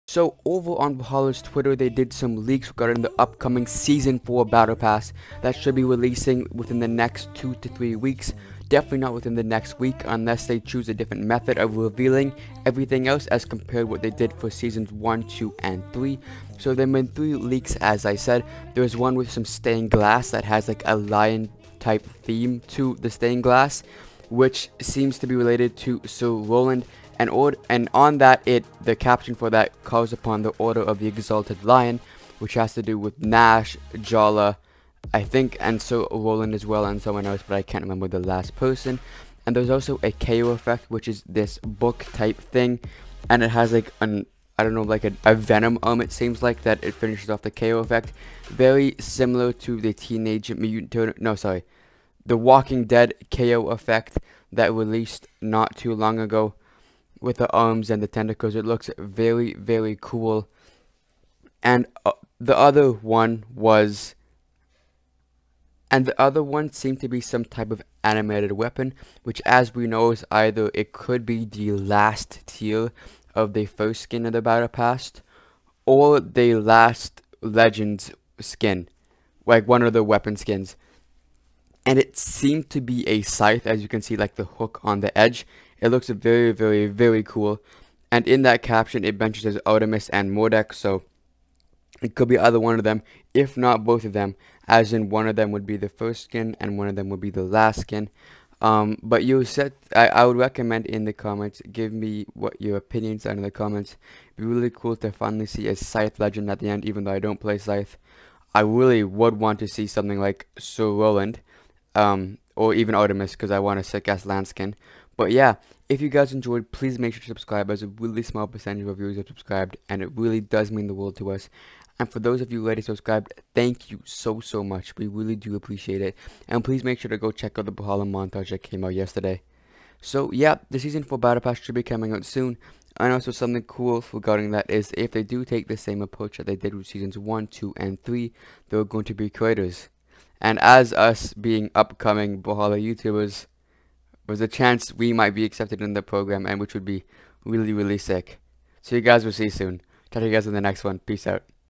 BRAWLHALLA SEASON FOUR BATTLE PASS LEAKS
In this video Mango talks about the reveals that have been made regarding the upcoming new season four battle pass in Brawlhalla.
Hope you enjoyed!